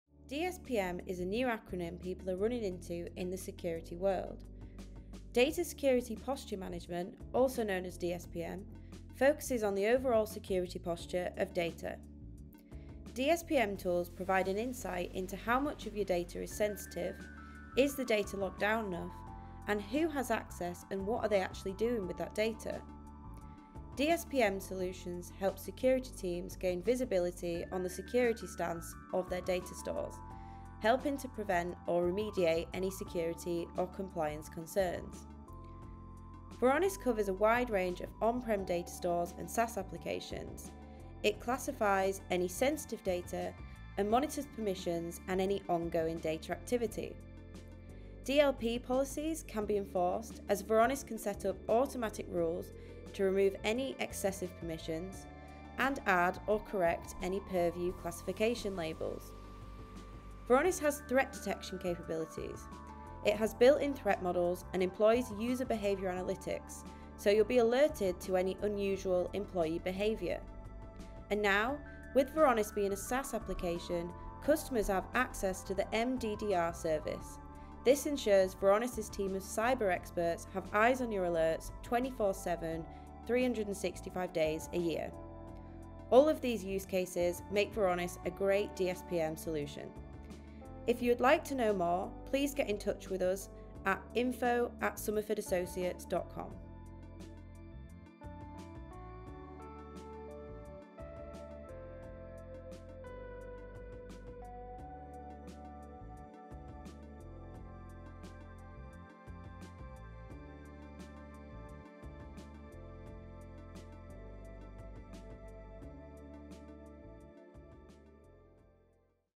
What is Data Security Posture Management (DSPM) and How Does it Work?
We answer what DSPM is and how securing sensitive data can be made easy with Varonis.